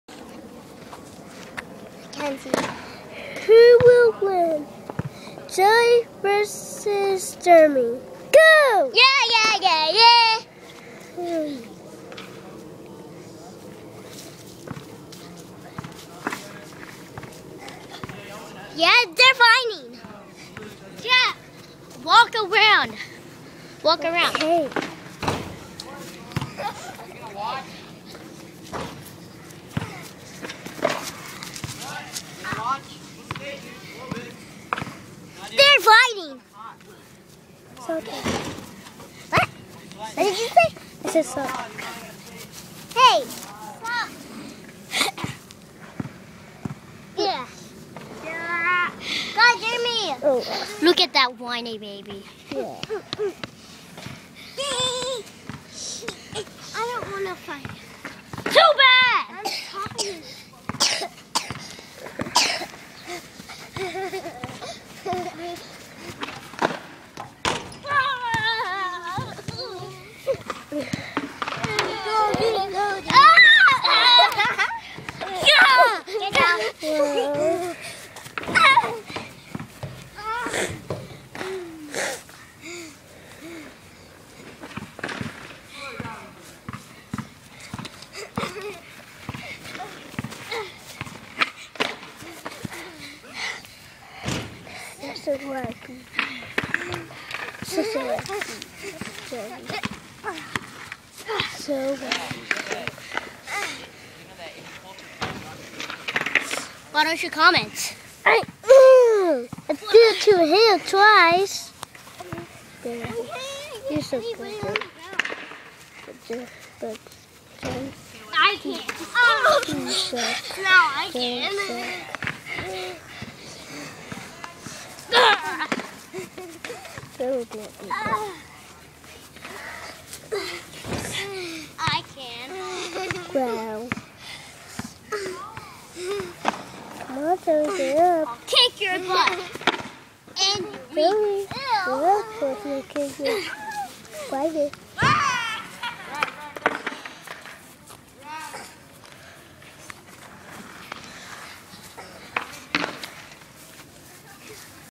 Who Will Win? Jeremy vs. Joey
Who will win is back! It's been so long since we made it, but it's back! This fight is made with:
Cameraman: Jack Wingler
Joey: Joey
Jayelldee02: Jayelldee02
Random person: Jade Dimalanta